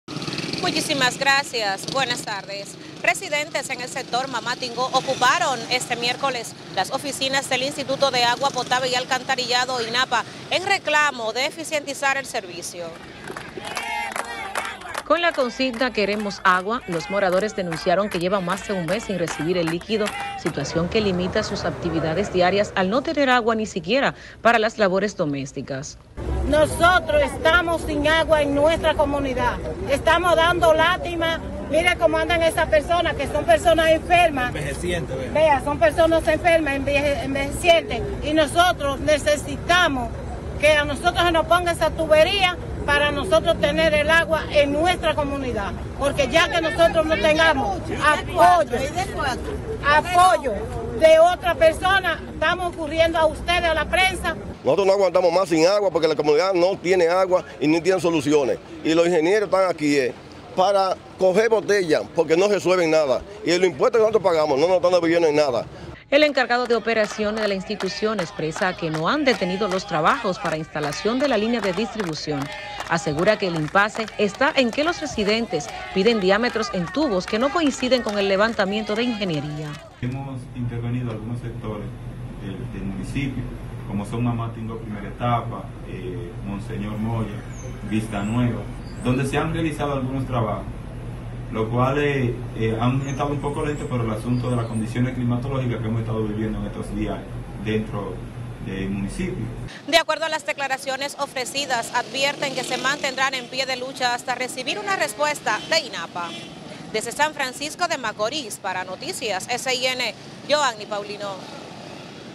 Residentes en sector Mamá Tingo exigen agua potable
Residentes en el sector Mamá Tingo ocuparon este miércoles las oficinas del Instituto de Aguas potables y Alcantarillado (INAPA) en reclamo de eficientizar el servicio.